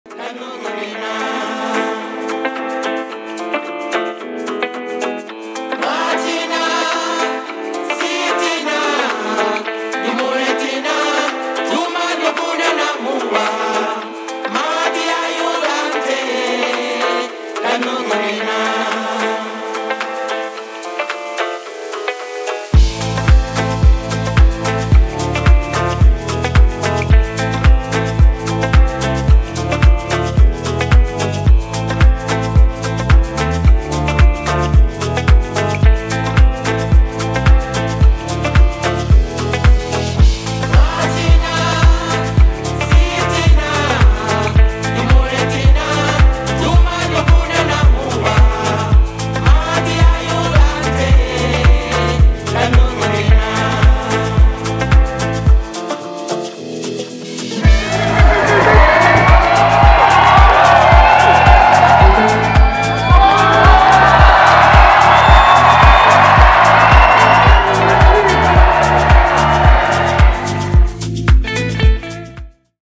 Synapson Live at Garorock 2022
Always so much pleasure and energy to live the Garorock Experience! Happy 25th anniversary! Let's have a look back at this fantastic concert.

Directed by Alex CRK